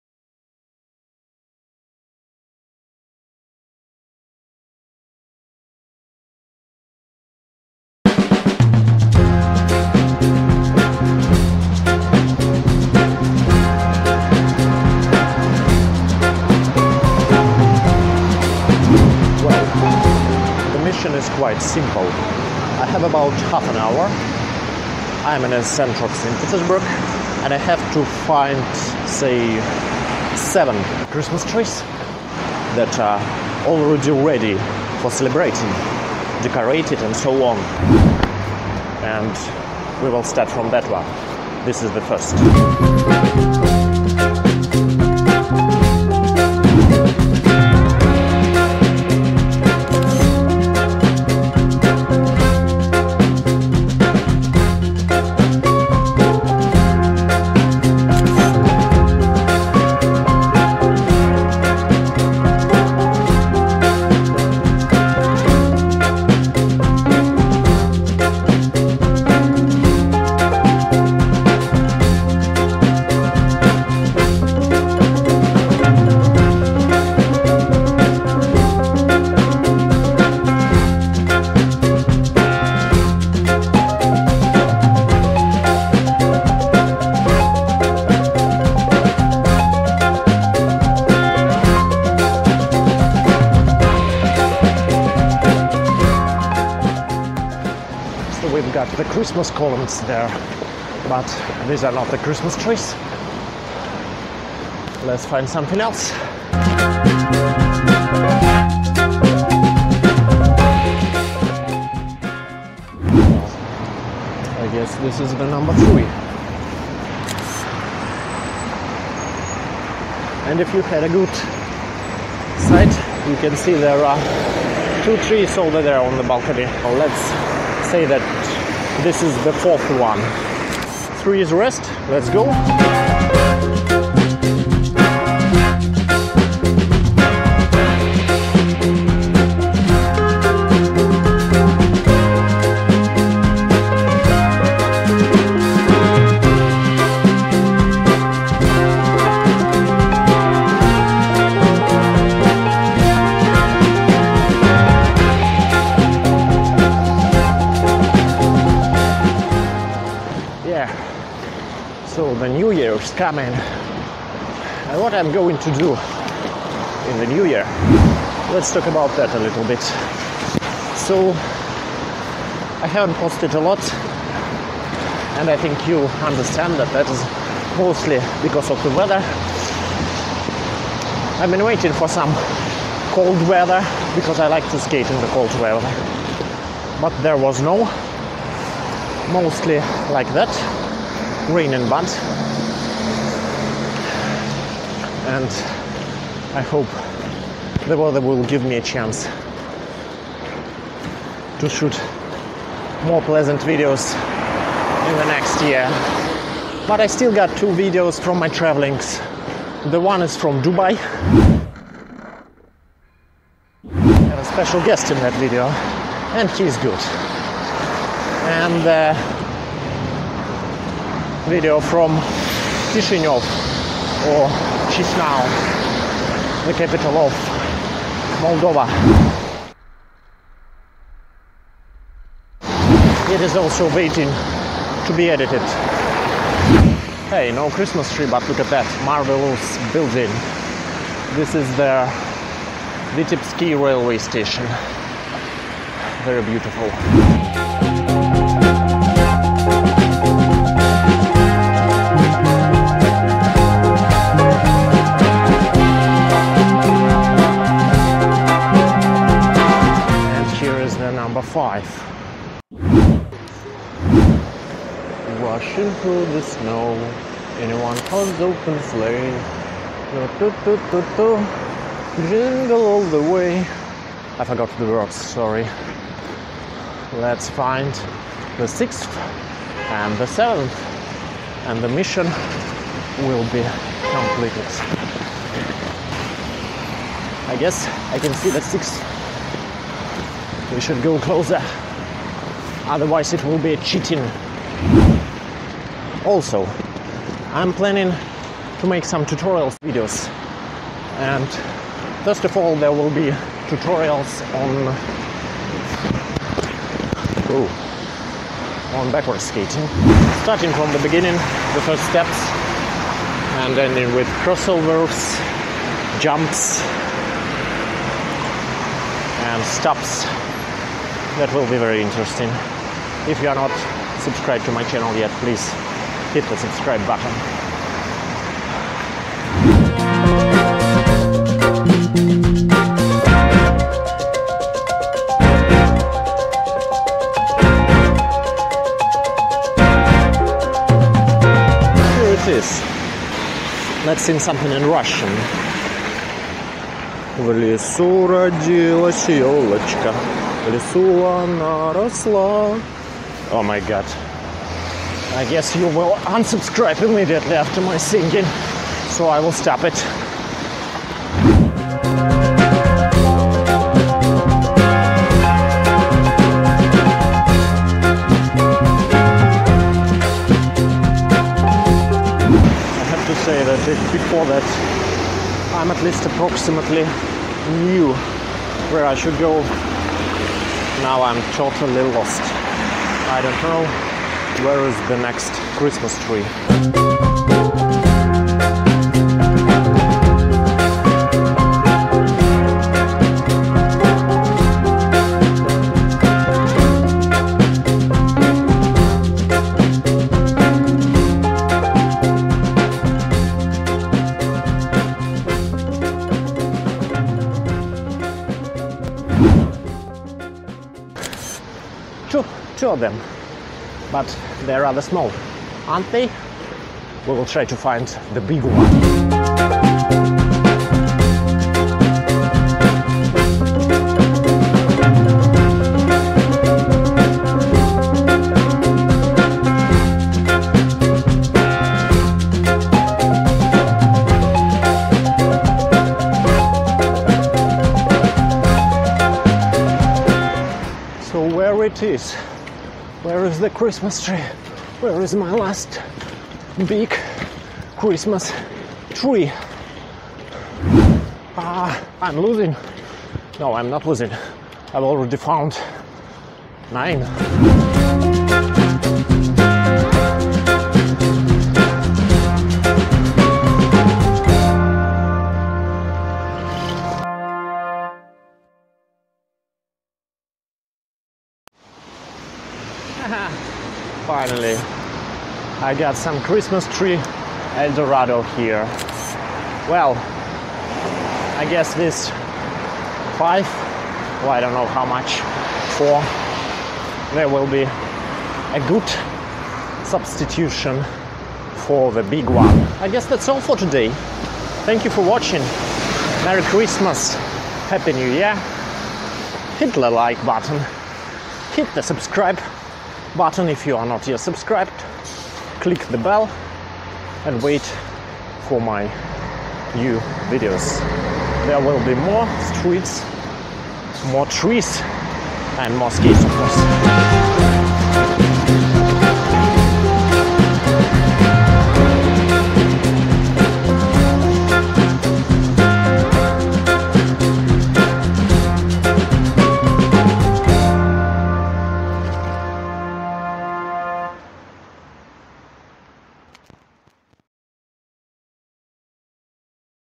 Inline skating hunt for Christmas Trees, New Year Plans etc 2020
Nik Zvi hunts for Christmas trees in the winter Saint-Petersburg. On inline skates Powerslide Next Supercruiser